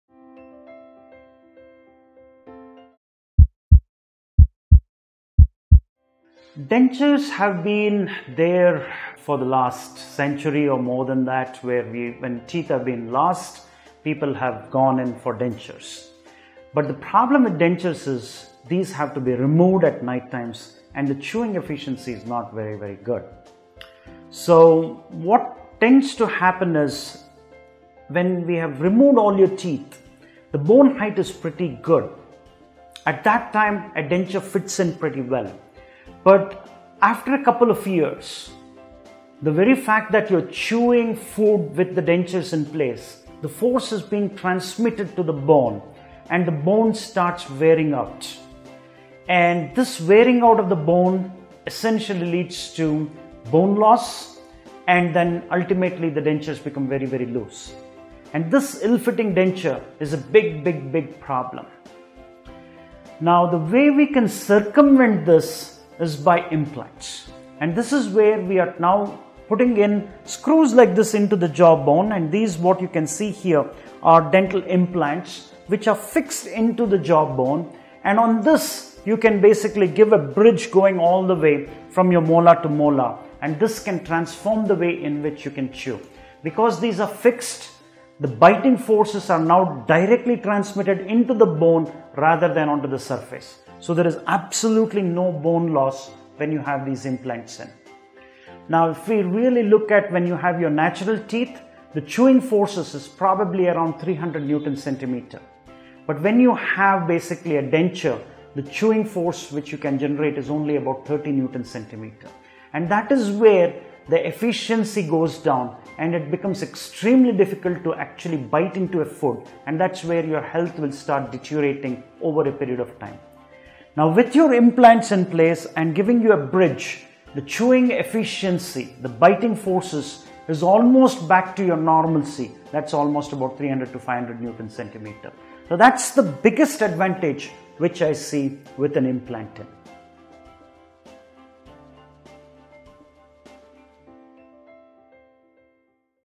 Inability to chew with Dentures Causes & Remedies - Dr. Girish Rao
Dentures have been there for the last century or more where teeth have been lost, people have gone in for dentures. But the problem with dentures is these have to be removed at night and the chewing efficacy is not very good. What tends to happen is when we have  removed all the  teeth, the bone  height  is pretty  good and the denture  fits  in pretty good but after a  couple of  years, the fact that  you  are chewing  food  with the dentures in place, the force is being transmitted to the bone and the bone starts  wearing off  and this  leads  to bone loss and ultimately the  dentures  become very loose . This ill-fitting  dentures is a big  problem and the way  we can circumvent this is we put in screws  in the  jaw  bone  and this is fixed into the jaw  bone and on this we can give a bridge from molar to  molar. This can transform the way you can chew. Because these are fixed, the biting force is transmitted onto the bone rather than to the surface. There is absolutely no bone loss when there is implants.  If  we look at  when we have  natural teeth, the  chewing force is  around 300 Newton centimeter and that is where the efficiency  goes  down and that is where your  health starts deteriorating over a period of time.  With  your implants in place  and  giving  you a  bridge  the chewing efficiency  or  the biting force  is  almost  back to your  normalcy, that is  about 300 to 500 Newton centimeter. That is the biggest advantage with the implant in.